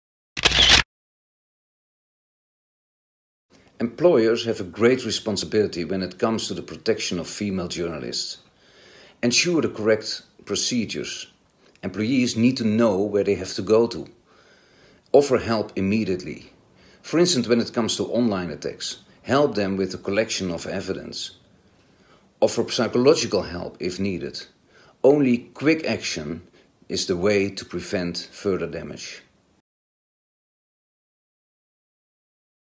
Media4Women 2022 | Gender Equality Champion | Peter ter Velde
During the Media4Women campaign we highlight Gender Equality Champions: people who lead the way in working towards gender equality in the media. One of our champions, Peter ter Velde, is project manager for the Dutch organisation PersVeilig (Press Safe), “We see that women journalists are attacked much more on the fact that they are women, than men are on them being men.”